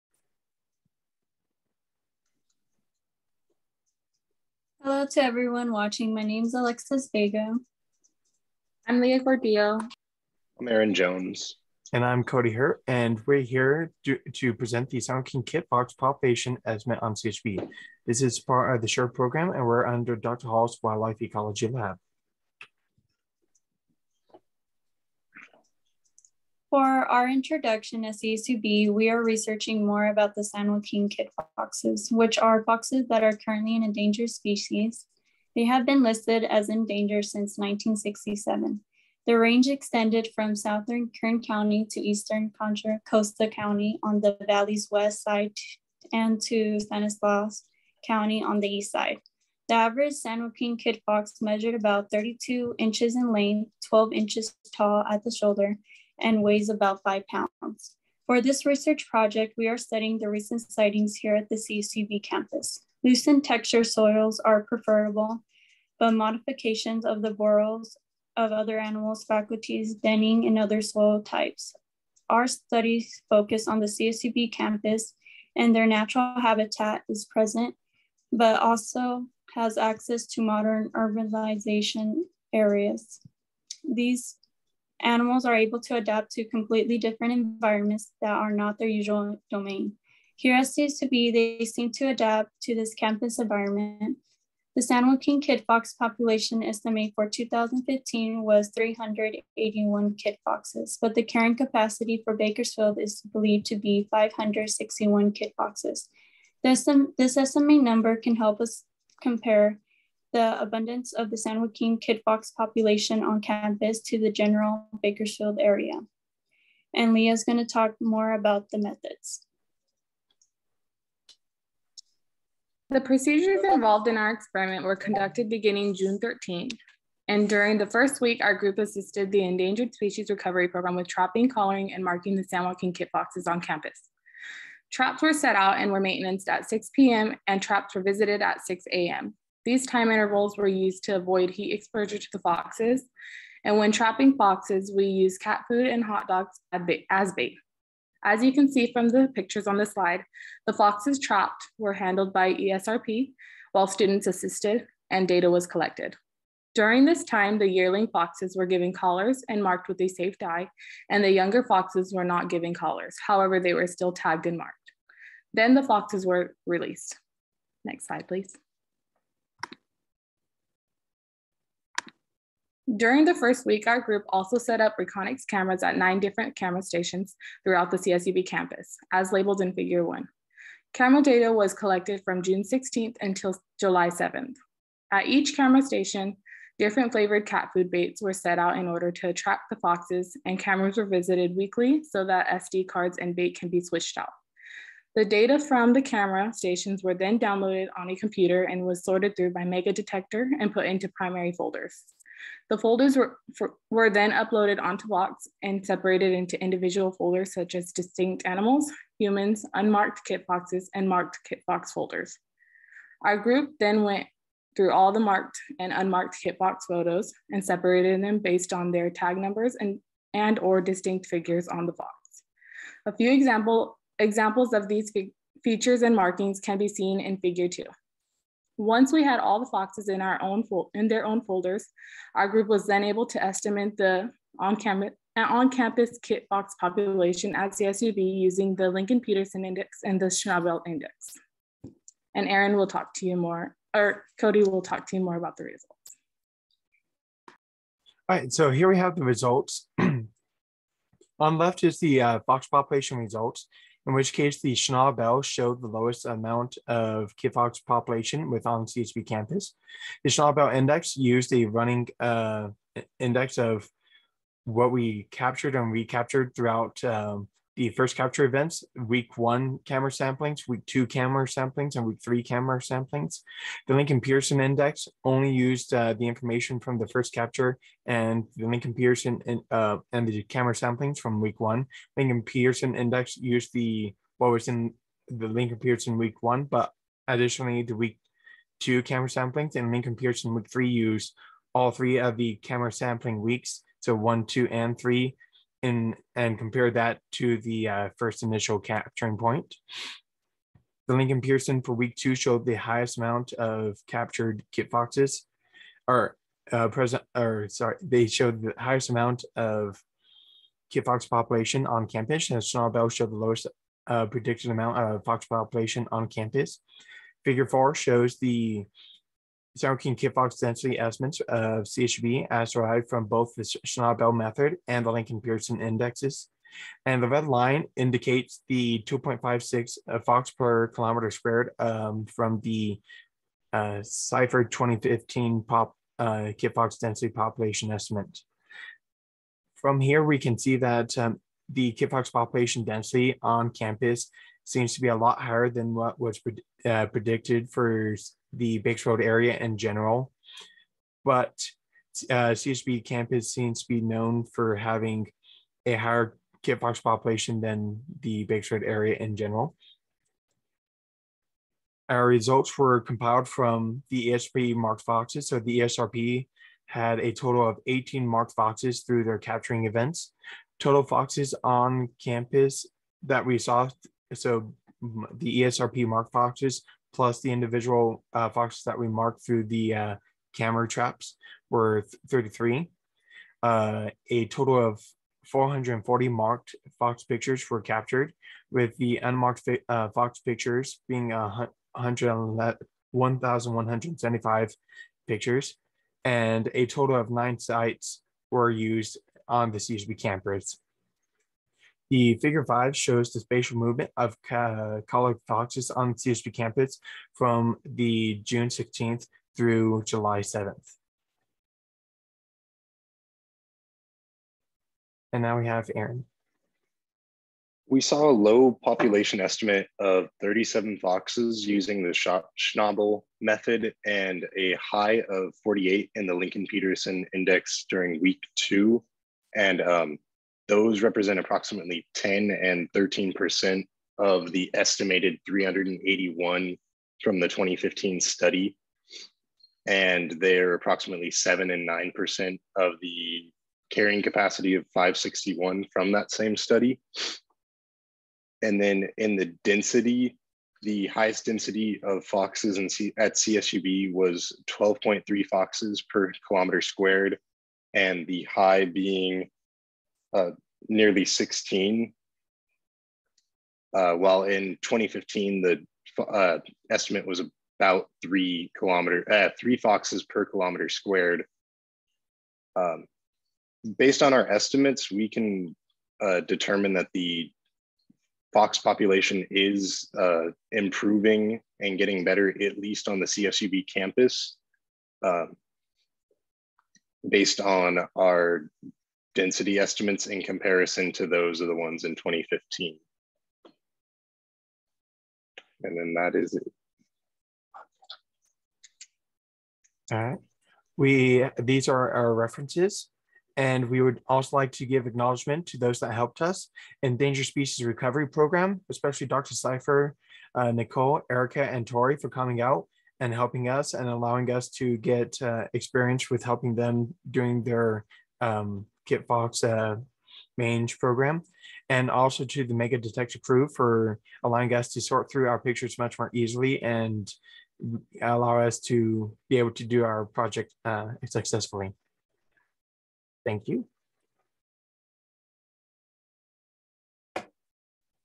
SURE 2022 | San Joaquin Kit Fox Population Estimate on CSUB
The Summer Undergraduate Research Experience program gives CSUB students the chance to participate in hands-on research. This is one of 12 projects for the 2022 SURE program.